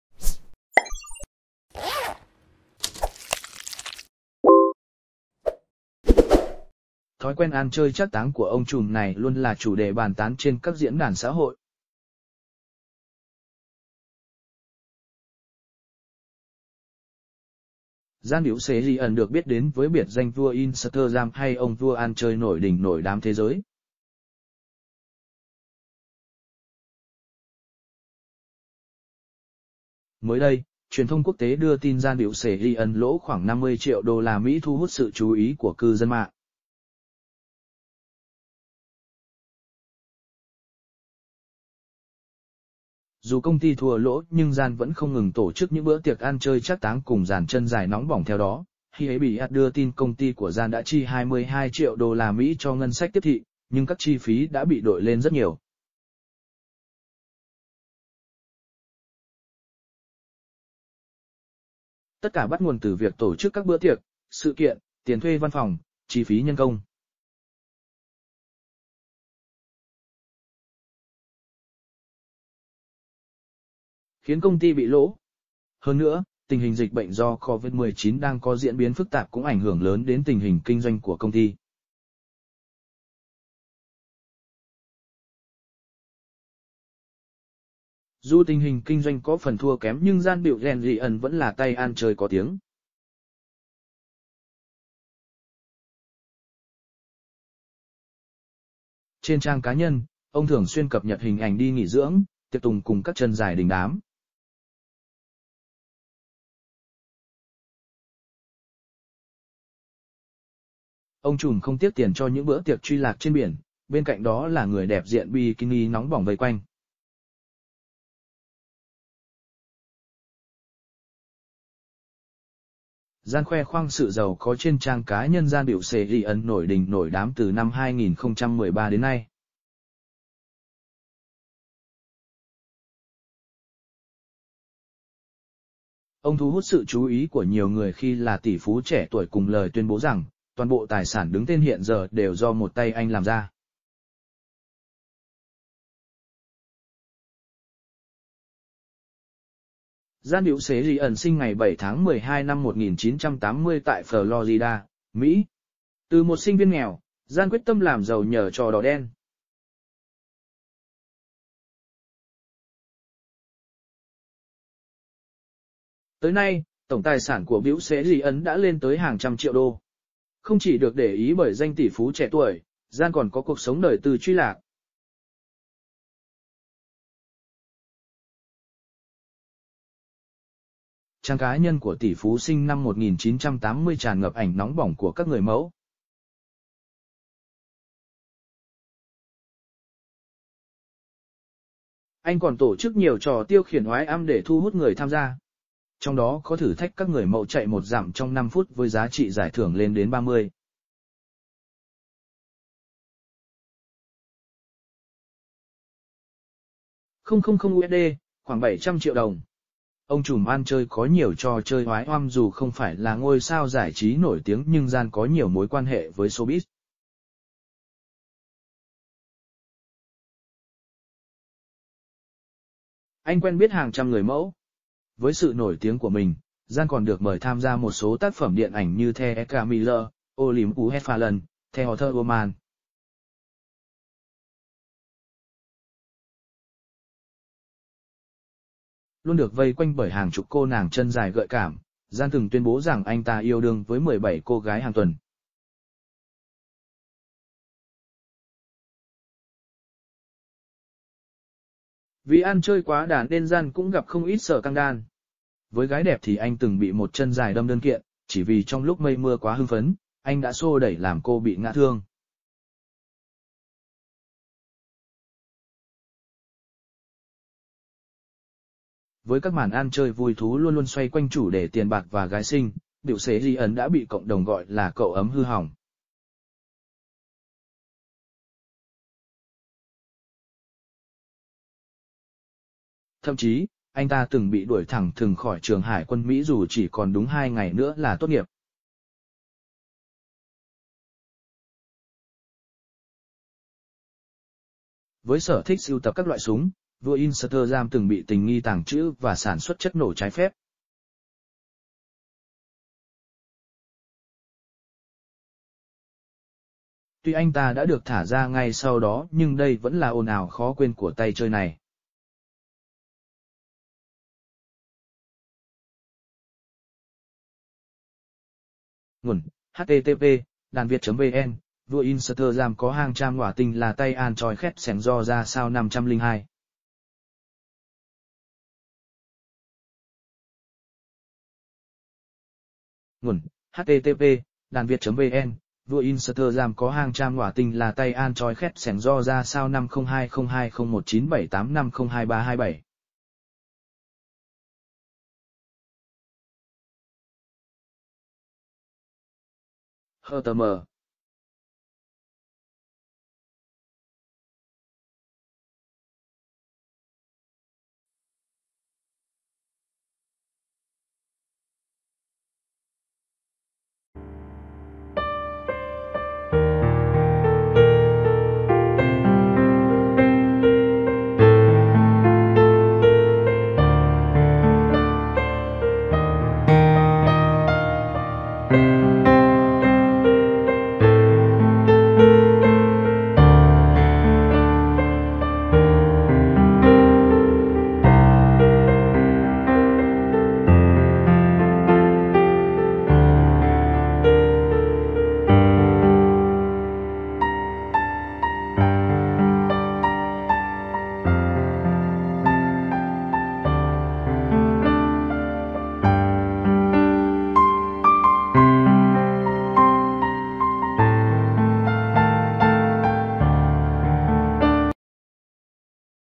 Vua Instagram có hàng trăm người tình, là tay ăn chơi khét tiếng giờ ra sao?
Thói quen ăn chơi trác táng của ông trùm này luôn là chủ đề bàn tán trên các diễn đàn xã hội.    Dan Bilzerian được biết đến với biệt danh "Vua Instagram" hay "Ông vua ăn chơi" nổi đình nổi đám thế giới. Mới đây, truyền thông quốc tế đưa tin Dan Bilzerian lỗ khoảng 50 triệu USD thu hút sự chú ý của cư dân mạng.  Dù công ty thua lỗ nhưng Dan vẫn không ngừng tổ chức những bữa tiệc ăn chơi trác táng cùng dàn chân dài nóng bỏng  Theo đó, Hypebeast đưa tin công ty của Dan đã chi 22 triệu USD cho ngân sách tiếp thị, nhưng các chi phí đã bị đội lên rất nhiều. Tất cả bắt nguồn từ việc tổ chức các bữa tiệc, sự kiện, tiền thuê văn phòng, chi phí nhân công... khiến công ty bị lỗ. Hơn nữa, tình hình dịch bệnh do Covid-19 đang có diễn biến phức tạp cũng ảnh hưởng lớn đến tình hình kinh doanh của công ty.  Dù tình hình kinh doanh có phần thua kém nhưng Dan Bilzenrian vẫn là tay ăn chơi có tiếng. Trên trang cá nhân, ông thường xuyên cập nhật hình ảnh đi nghỉ dưỡng, tiệc tùng cùng các chân dài đình đám. Ông trùm không tiếc tiền cho những bữa tiệc trụy lạc trên biển, bên cạnh đó là người đẹp diện bikini nóng bỏng vây quanh.  Dan khoe khoang sự giàu có trên trang cá nhân  Dan Bilzerian nổi đình nổi đám từ năm 2013 đến nay. Ông thu hút sự chú ý của nhiều người khi là tỷ phú trẻ tuổi cùng lời tuyên bố rằng, toàn bộ tài sản đứng tên hiện giờ đều do một tay anh làm ra. Dan Bilzerian sinh ngày 7/12/1980 tại Florida, Mỹ. Từ một sinh viên nghèo, Dan quyết tâm làm giàu nhờ trò đỏ đen. Tới nay, tổng tài sản của Bilzerian đã lên tới hàng trăm triệu đô.  Không chỉ được để ý bởi danh tỷ phú trẻ tuổi, Dan còn có cuộc sống đời tư trụy lạc. Trang cá nhân của tỷ phú sinh năm 1980 tràn ngập ảnh nóng bỏng của các người mẫu. Anh còn tổ chức nhiều trò tiêu khiển oái ăm để thu hút người tham gia. Trong đó có thử thách các người mẫu chạy 1 dặm trong 5 phút với giá trị giải thưởng lên đến 30.000 USD (khoảng 700 triệu đồng).  "Ông trùm ăn chơi" có nhiều trò chơi oái oăm   Dù không phải là ngôi sao giải trí nổi tiếng nhưng Dan có nhiều mối quan hệ với showbiz. Anh quen biết hàng trăm người mẫu. Với sự nổi tiếng của mình, Dan còn được mời tham gia một số tác phẩm điện ảnh như The Equalizer, Olympus Has Fallen, The Other Woman.  Luôn được vây quanh bởi hàng chục cô nàng chân dài gợi cảm, Dan từng tuyên bố rằng anh ta "yêu đương" với 17 cô gái hàng tuần. Vì ăn chơi quá đà nên Dan cũng gặp không ít scandal. Với gái đẹp thì anh từng bị một chân dài đâm đơn kiện, chỉ vì trong lúc mây mưa quá hưng phấn, anh đã xô đẩy làm cô bị ngã thương.  Với các màn ăn chơi vui thú luôn luôn xoay quanh chủ đề tiền bạc và gái xinh, Bilzerian đã bị cộng đồng gọi là cậu ấm hư hỏng. Thậm chí, anh ta từng bị đuổi thẳng thừng khỏi trường hải quân Mỹ dù chỉ còn đúng 2 ngày nữa là tốt nghiệp.  Với sở thích sưu tập các loại súng, vua Instagram từng bị tình nghi tàng trữ và sản xuất chất nổ trái phép. Tuy anh ta đã được thả ra ngay sau đó nhưng đây vẫn là ồn ào khó quên của tay chơi này. Nguồn